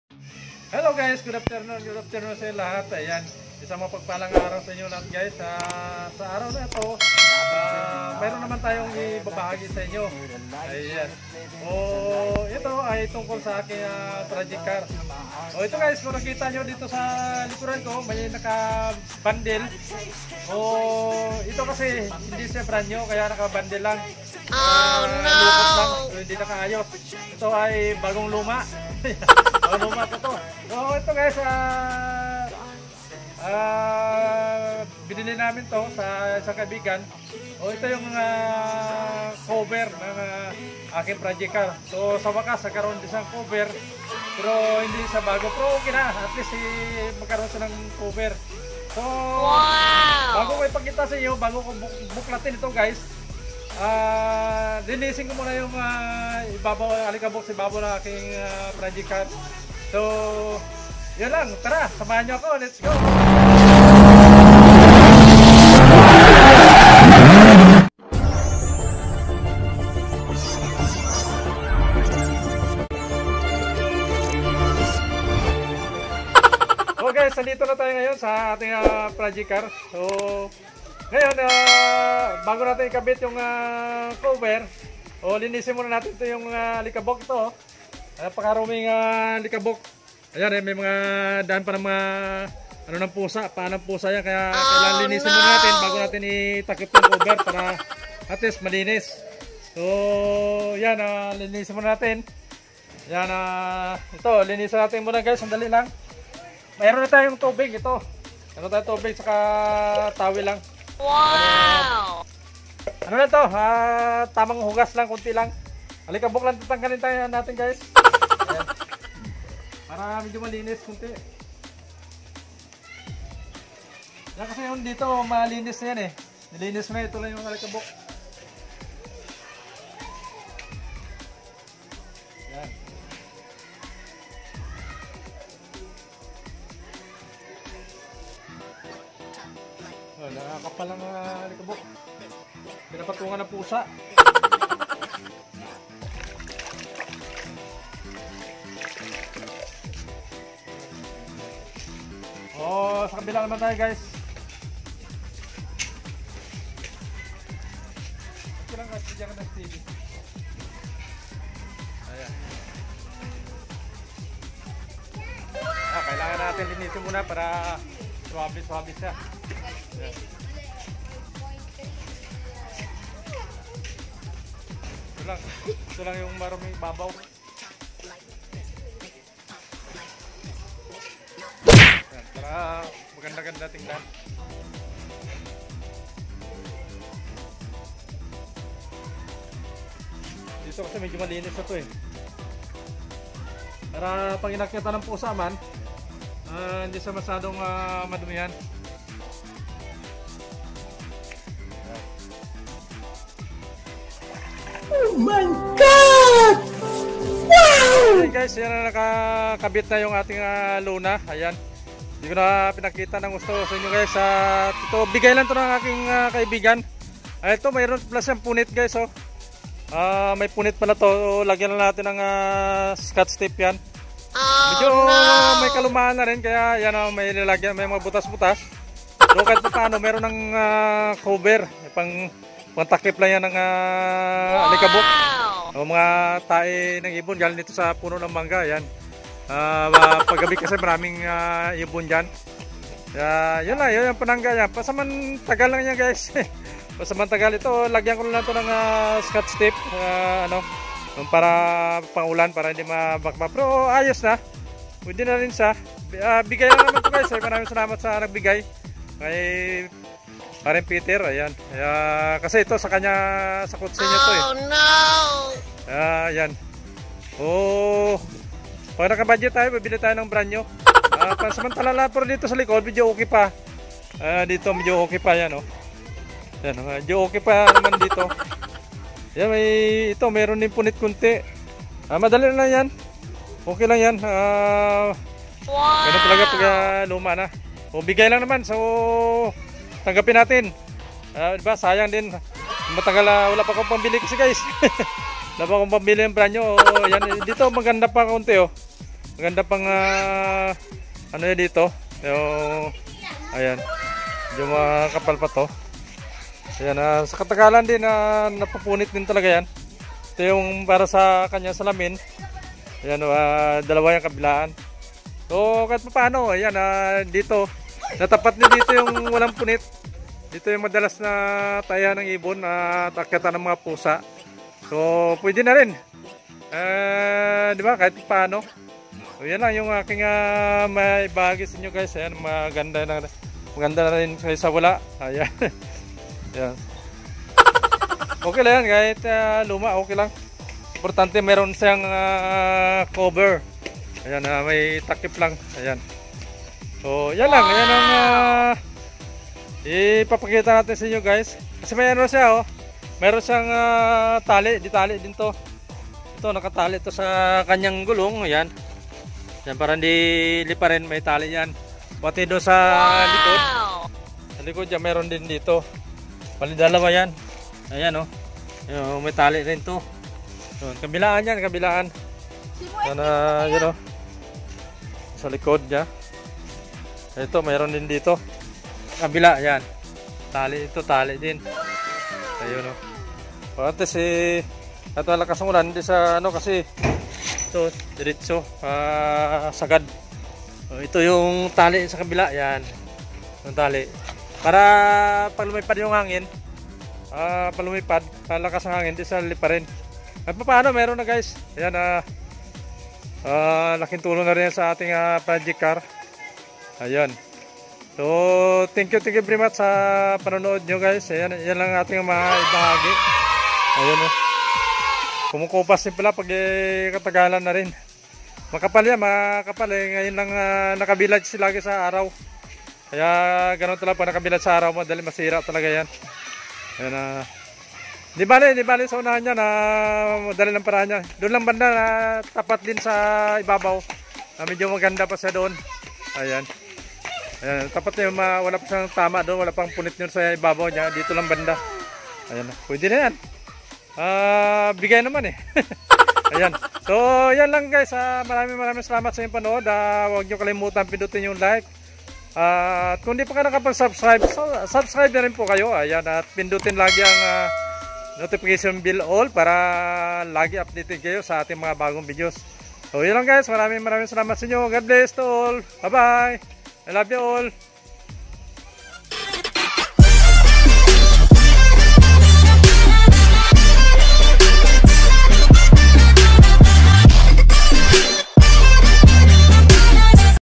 SA WAKAS NAGKAROON DIN NG COVER ANG PROJECT CAR KO! (@Joefel tv)
may nagbigay ng car cover para sa project car ko maraming salamat po sa nag bigay god bless idol! (@Joefel tv)